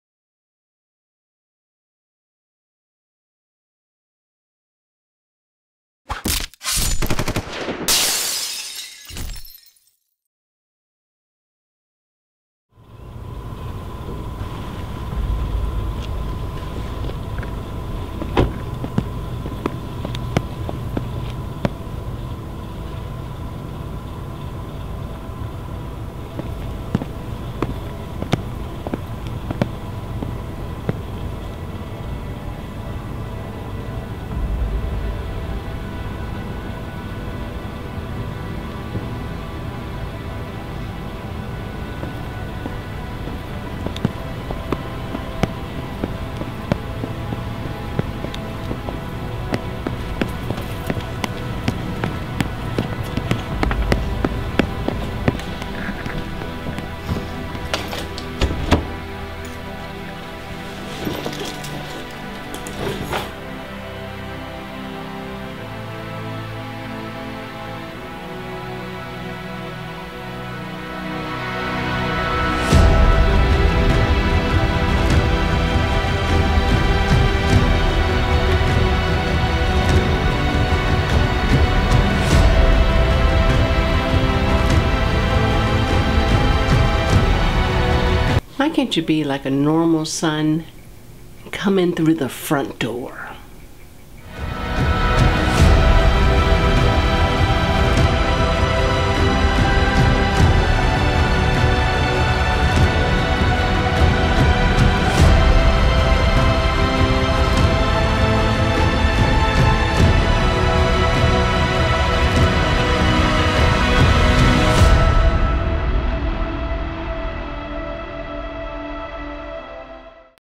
An Unexpected Visitor (My RØDE Reel)
Woman gets home after a long day, but is she alone?

This video was made for the My RØDE Reel contest.